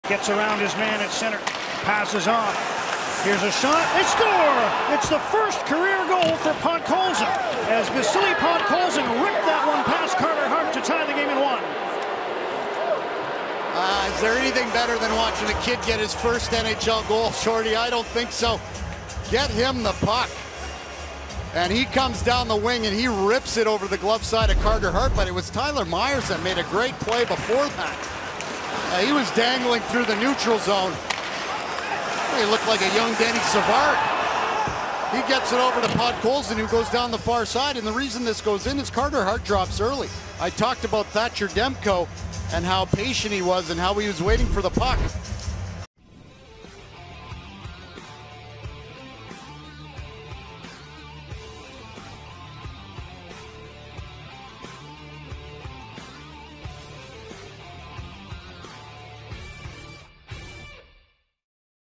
Vasily Podkolzin Snipes Puck Past Carter Hart For First Career NHL Goal
Vancouver Canucks rookie Vasily Podkolzin ripped a shot past Carter Hart to score his first career NHL goal.